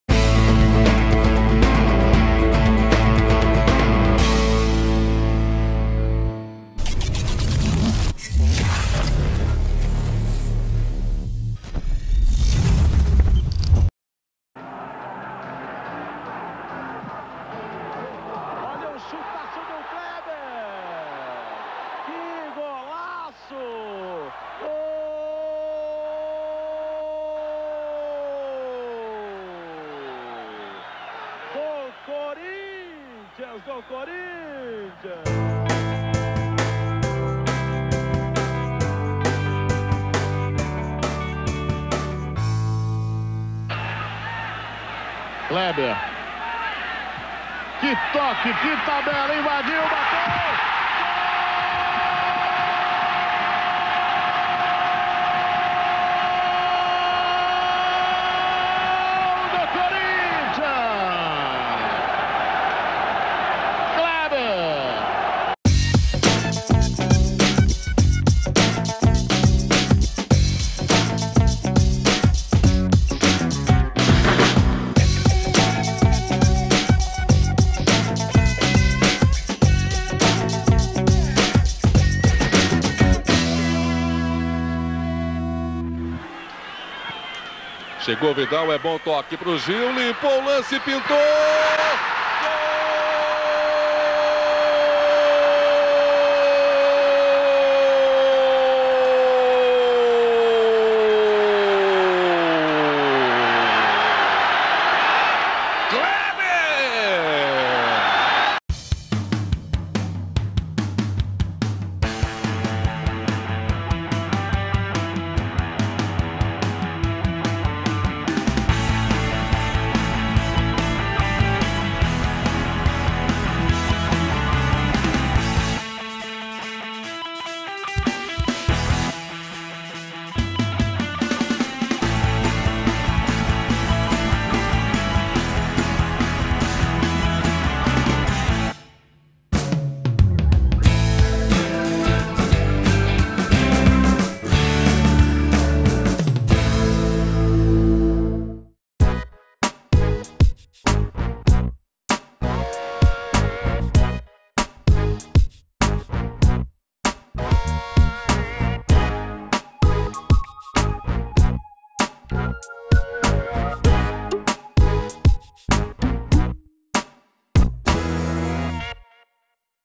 Lateral Kleber | Gols pelo Corinthians
Os gols do lateral esquerdo Kleber com a camisa do Corinthians. Comente, participe, nos ajude a divulgar.